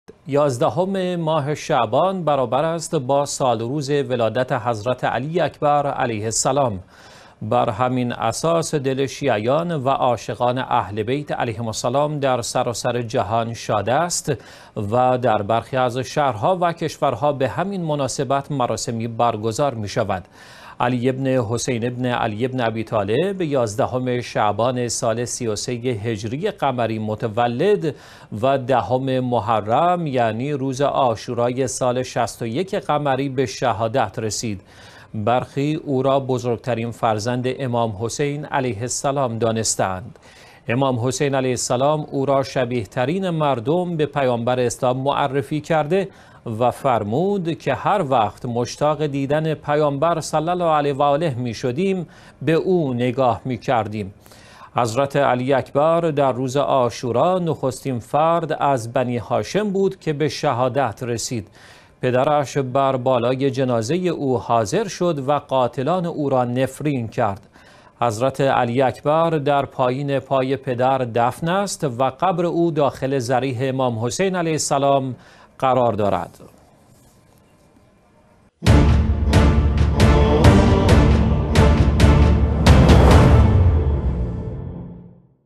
۱۱ شعبان .. سالروز ولادت حضرت علی اکبر علیه السلام
یازدهم ماه شعبان برابر است با سالروز ولادت حضرت علی اکبر علیه السلام.
بر همین اساس دل شیعیان و عاشقان اهل بیت علیهم السلام در سراسر جهان، شاد است و در برخی از شهرها و کشورها به همین مناسبت مراسمی برگزار می شود.
علی بن حسین بن علی بن ابی‌طالب یازدهم شعبان سال ۳۳ هجری قمری متولد و دهم محرم یعنی روز عاشورا سال ۶۱ قمری به شهادت رسید.
برخی او را بزرگ‌ترین فرزند امام حسین علیه السلام دانسته‌اند. امام حسین علیه السلام او را شبیه‌ترین مردم به پیامبر اسلام معرفی کرده و فرمود که هر وقت مشتاق دیدن پیامبر صلی الله علیه و آله می‌شدیم، به او نگاه می‌کردیم.
حضرت علی‌اکبر در روز عاشورا نخستین فرد از بنی‌هاشم بود که به شهادت رسید. پدرش بر بالای جنازه او حاضر شد و قاتلان او را نفرین کرد.
حضرت علی‌اکبر در پایین پای پدر دفن است و قبر او داخل ضریح امام حسین علیه السلام قرار دارد.